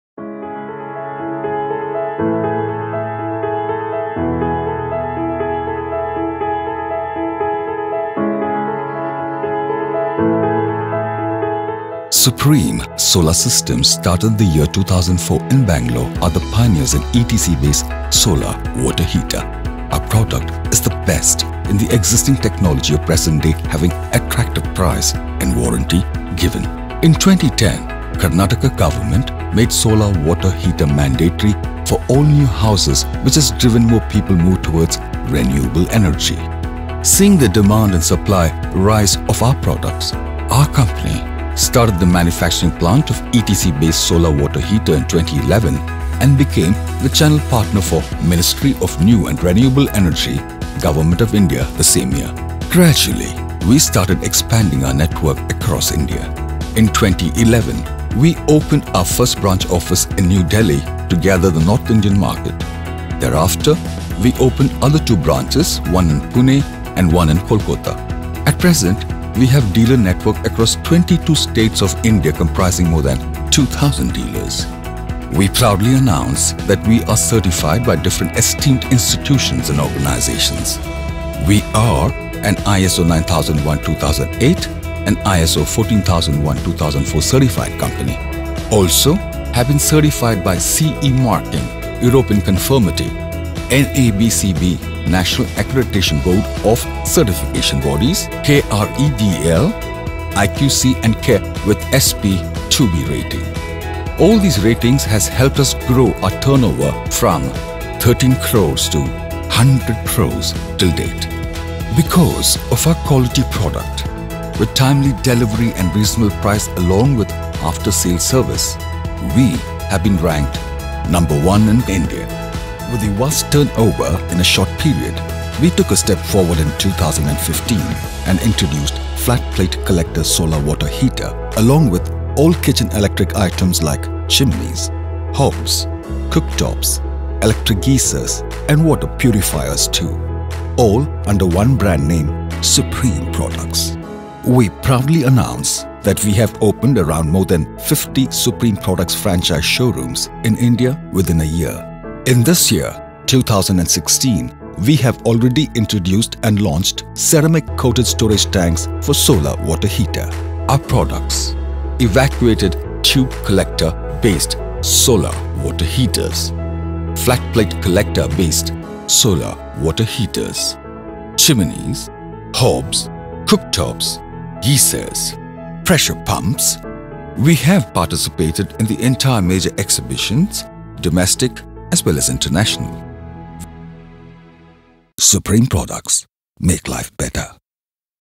Leading Solar Water Heaters Manufacturers in India.
Supreme Solar Projects Pvt. Ltd. is a Solar Water Heating Systems Manufacturing Company based in Bengaluru, Karnataka, India. It is a Leading Manufacturer & Exporter of Solar Water Heaters Systems all over India.
We recently launched the Glass Lined Solar Water Heating Systems into the market. It is an internationally renowned technology in Solar Water Heating   Industry to provide clean hot water.
The high quality international standard glass coating keeps the tank free of rust and corrosion.